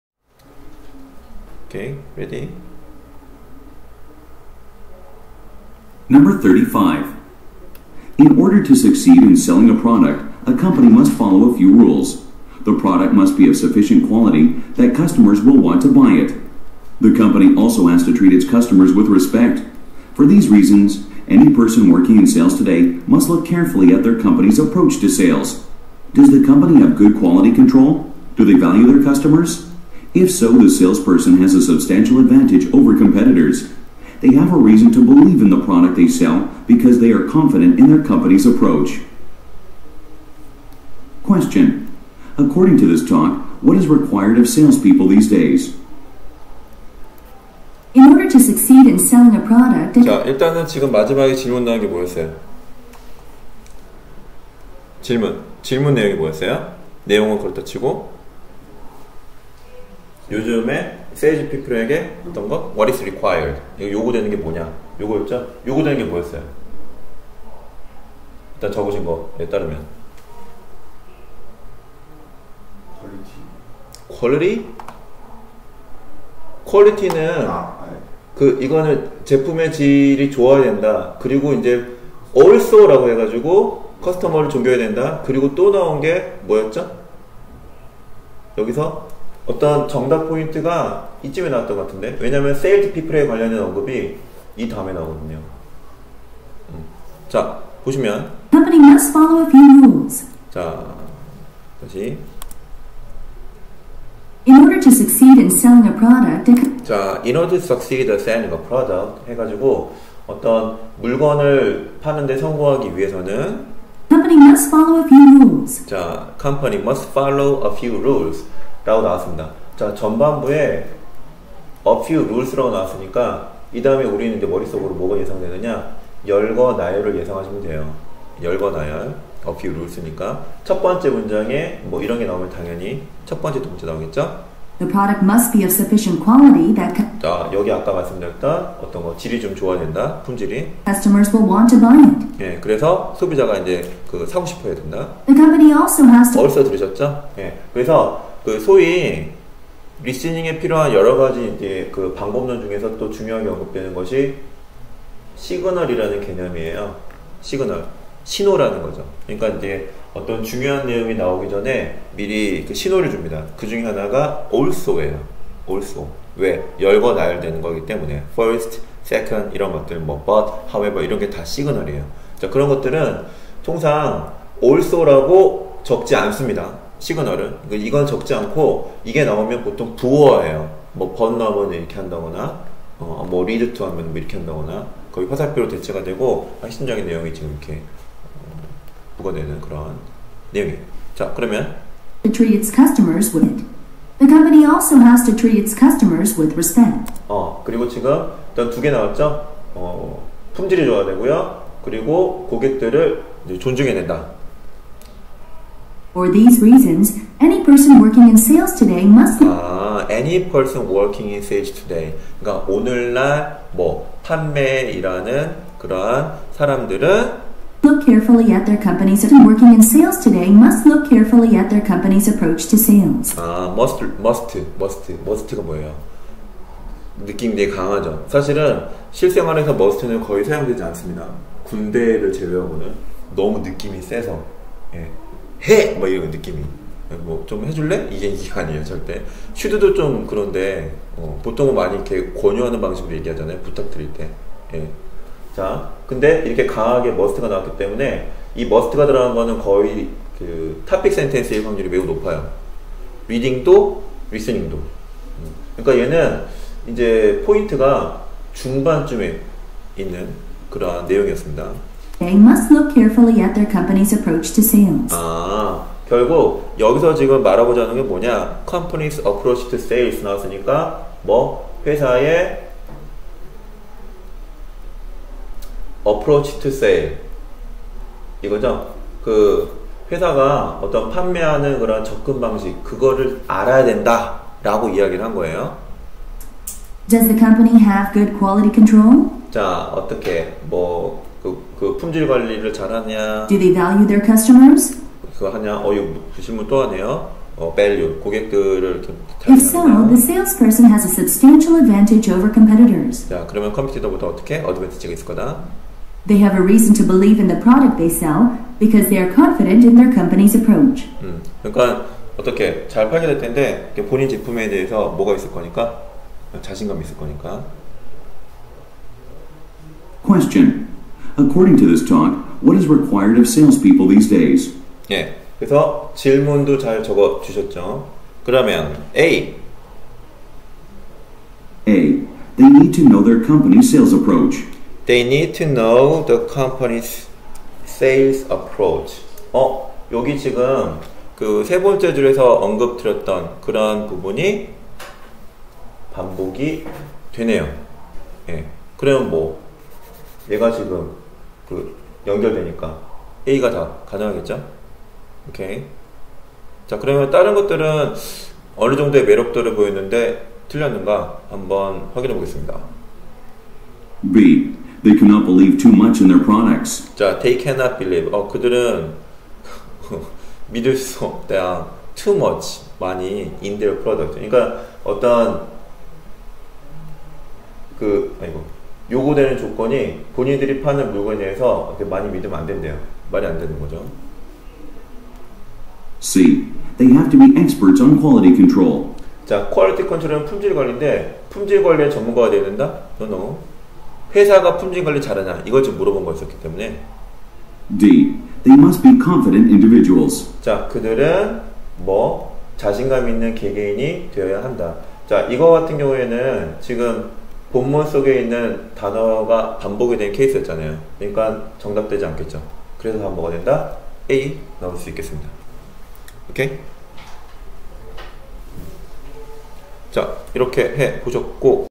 텝스 청해 파트 4 Q 35 A 1 Day 03 곽영어
다니엘샘의 뉴텝스 모의고사 A 1 해설강의입니다.